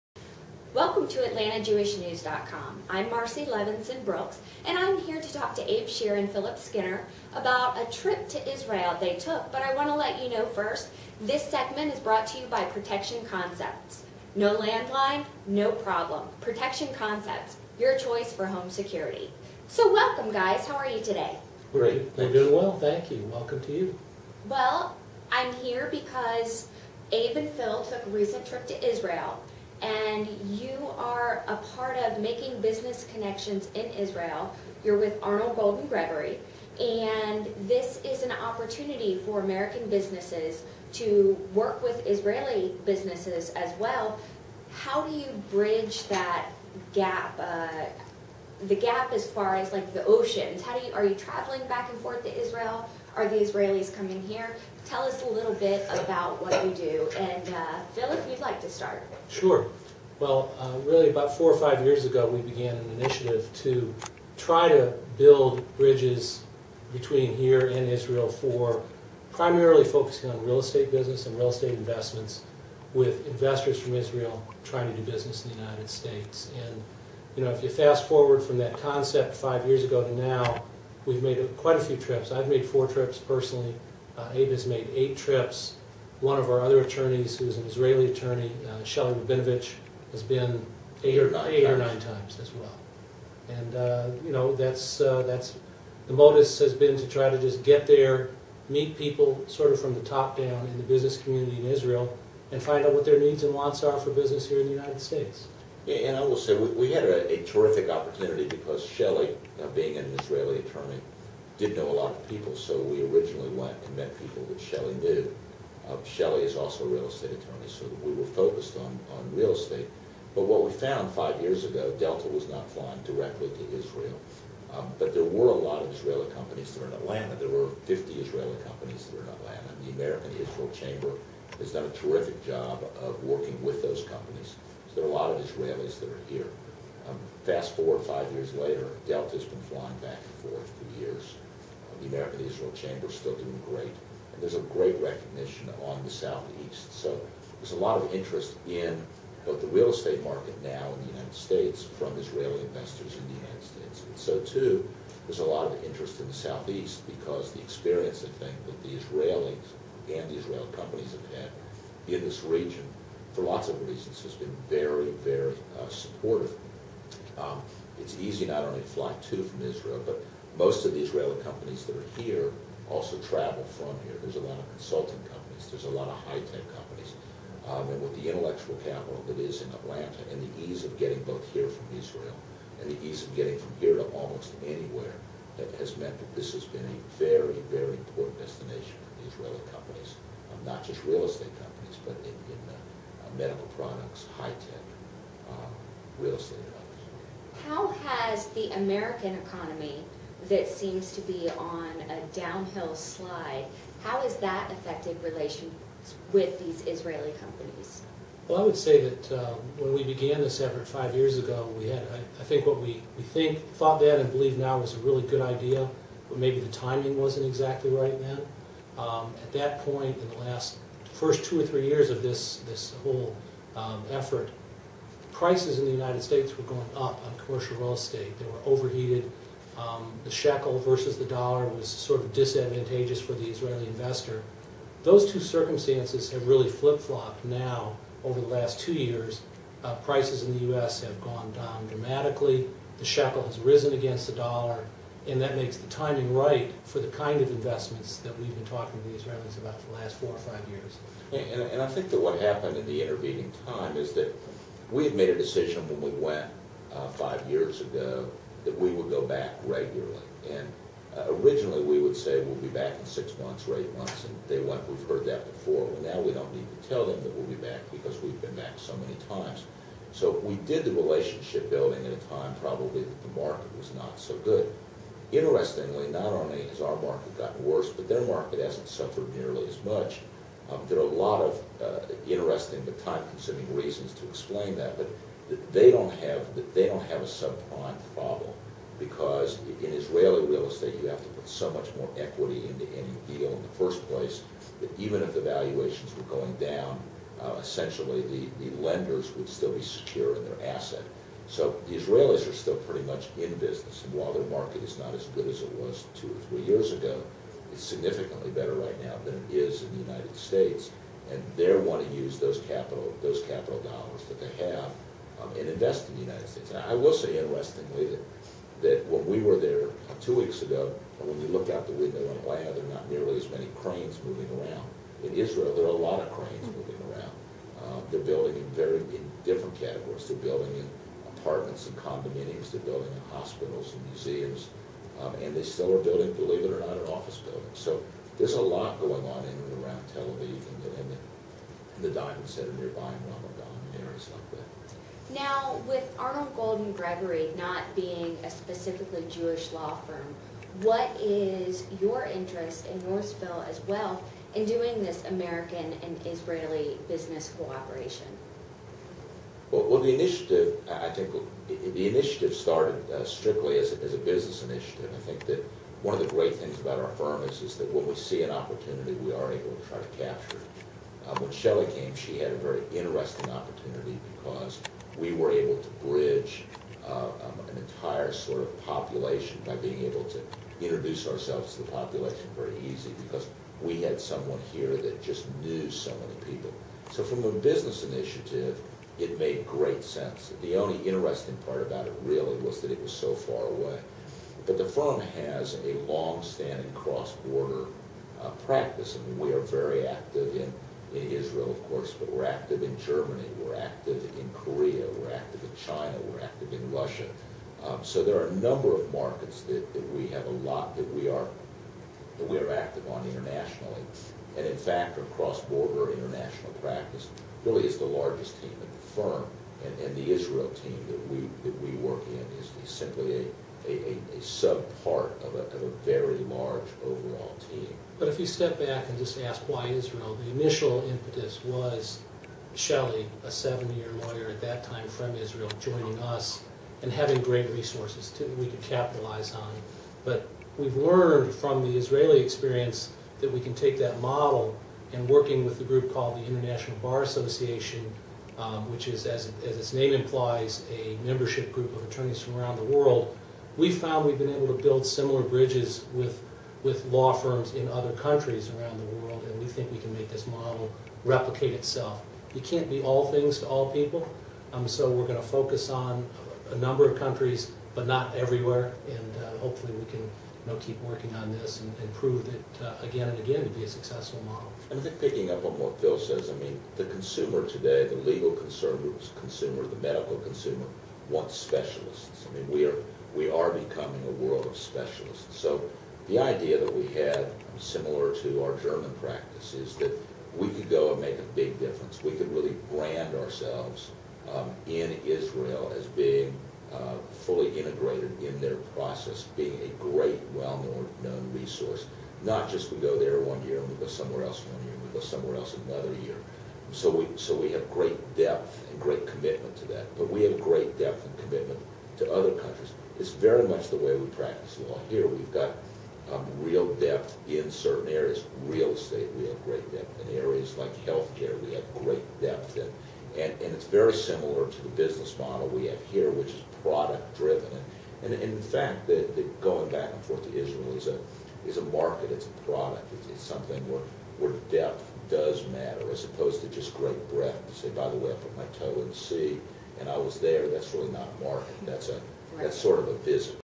AmericanIsraelibusiness_0001.wmv
American lawyers Abe Schear and Phil Skinner of Arnall Golden Gregory in Atlanta discuss American and Israeli business partnerships in Atlanta and around the world. Each of the partnerships they said, helps build the economies of the respective countries. Hosted for AtlantaJewishNews.com by Marcy J. Levinson-Brooks. Atlanta, Feb. 2010.